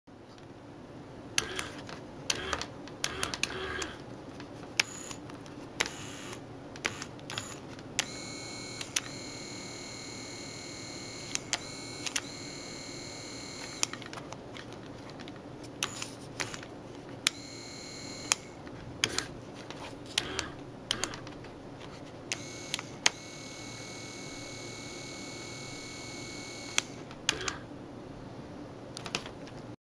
Robot Arm built with Photocopier Parts, version 0.01
I'm building a robot arm from some photocopier motors for my son. This is version 0.01 - hopefully many more iteration to come. The plan is to not use an Arduino - just plan old motors, switches, battery and cardboard.